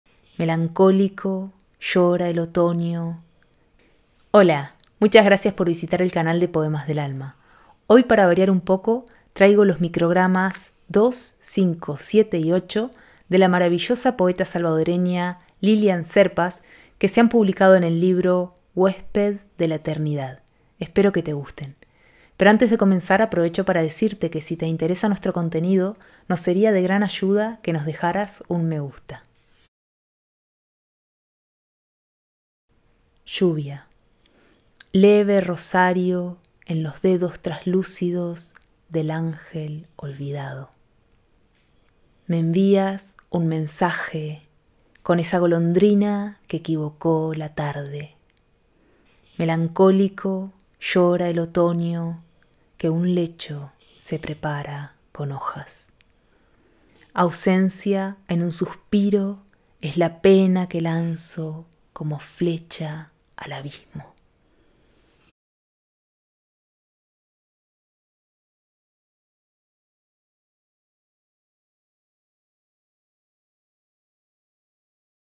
4 microgramas de Lilian Serpas
Leemos los microgramas 2, 5, 7 y 8 de Lilian Serpas.